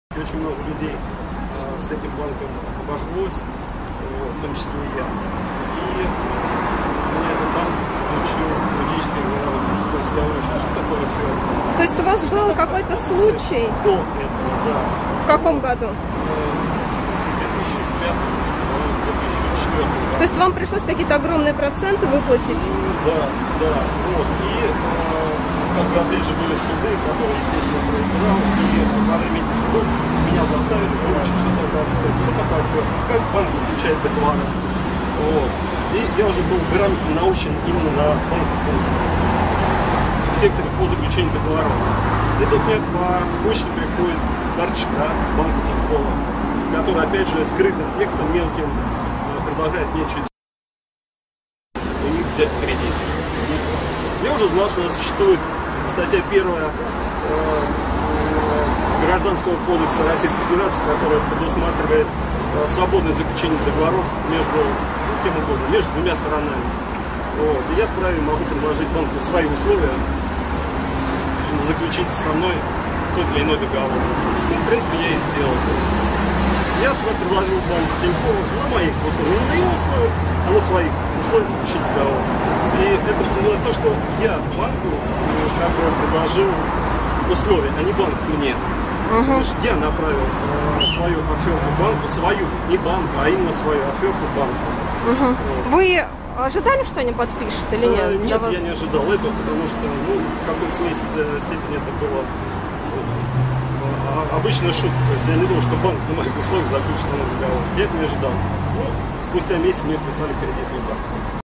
Воронежец требует с банка 24 миллиона рублей
Он требует от банка «Тинькофф Кредитные системы» исполнить обязательства -- компенсировать нарушения кредитного договора, условия которого предложил он сам. Воронежец направил в юридическую службу банка для урегулирования спора в досудебном порядке претензию, в которой указал сумму в 24 миллиона рублей. Претензия основывается на том, что банк нарушил условия кредитного договора. Подписав договор, банк согласился взять на себя обязательства, предложенные Алексеевым. Условия кредита из анкеты-заявления, прописанные в документе мелким шрифтом, клиент изменил на свои.